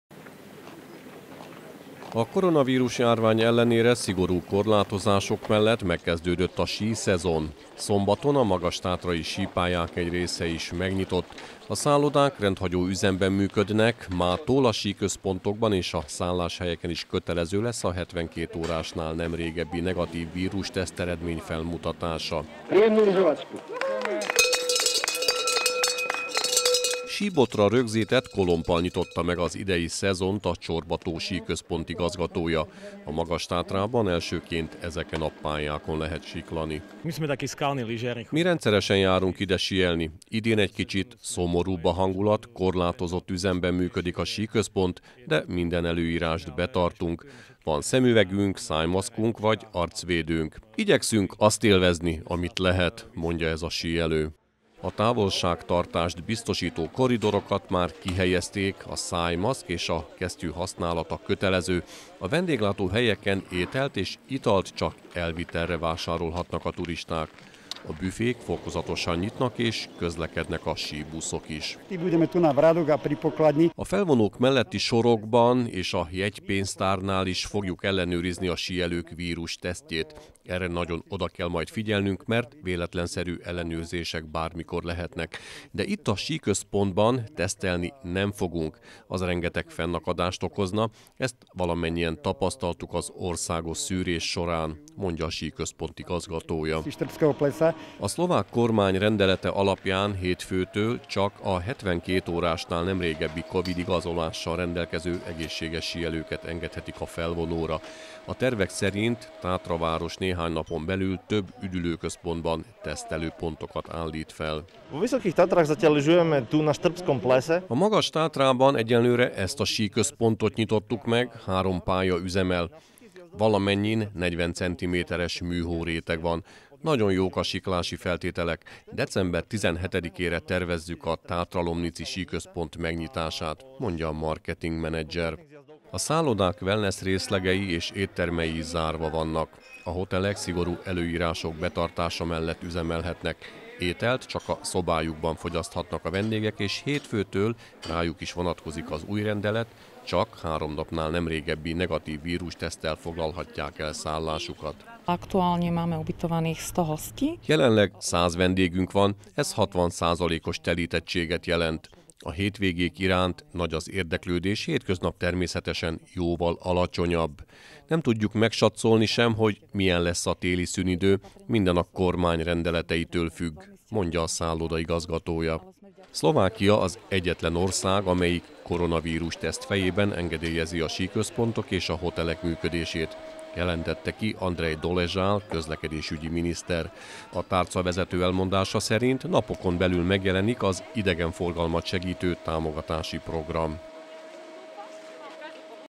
Korlátozásokkal indult a síszezon a Magas-Tátrában
Csorba-tó. A koronavírus-járvány ellenére, szigorú korlátozások mellett
megkezdődött a síszezon Szlovákiában. Ma a magas-tátrai sípályák egy része is
megnyitott. A szállodák rendhagyó üzemben működnek. Hétfőtől a
síközpontokban és a szálláshelyeken is kötelező lesz a 72 órásnál nem régebbi
negatív vírusteszteredmény felmutatása.